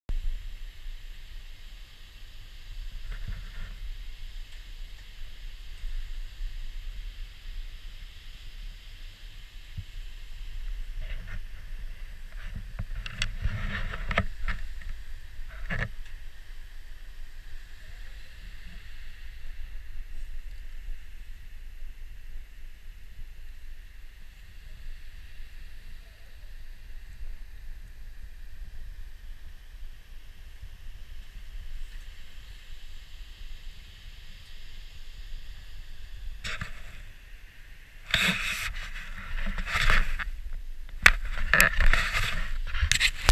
Skogafoss, Iceland from the top
from our March 2016 trip. we witnessed the Northern Lights that night. Freakin' magical!